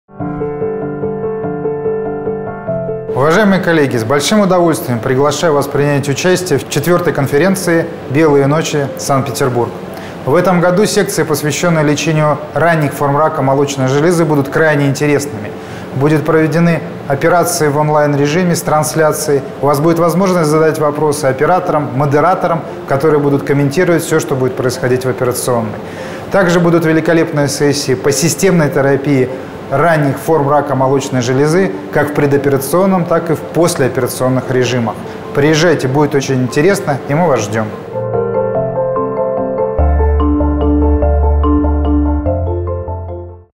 Криворотько Петр Владимирович - Приглашение на Петербургский онкологический форум "Белые ночи 2018"
Приглашение к участию в IV Петербургском международном онкологическом Форуме "Белые ночи 2018"